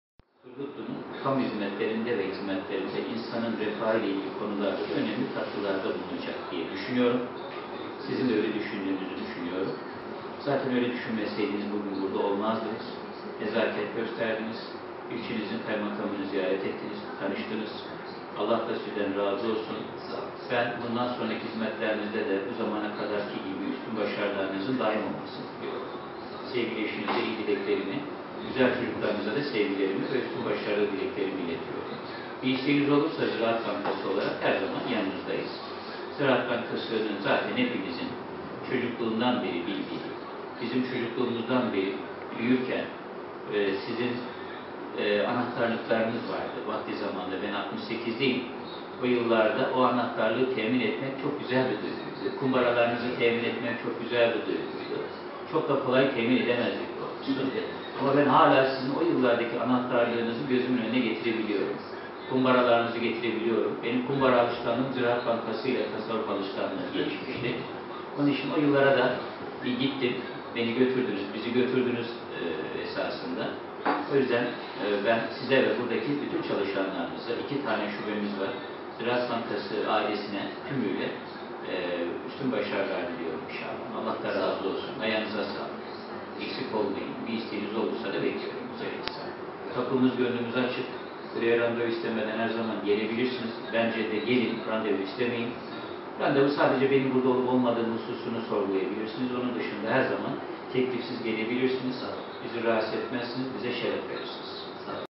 Turgutlu kaymakamı Uğur Turan'a ziyaret
Ziraat Bankası yeni müdüründen Kaymakam Turan’a ziyaret
T.C. ZİRAAT Bankası Turgutlu  7 Eylül Şubesi’ne yeni atanan  Erhan Kaya, bugün Turgutlu Kaymakamı Uğur Turan‘ı makamında nezaket ziyaretinde bulundu. Güzel diyalogların yaşandığı nezaket ziyaretinde Kaymakam Turan Turgutlu’nun ekonomik yapısında bilgiler verdi. Turan , Müdür Kaya’nın  nazik ziyaretinden dolayı teşekkür ederek, çalışmalarında üstün başarılar diledi.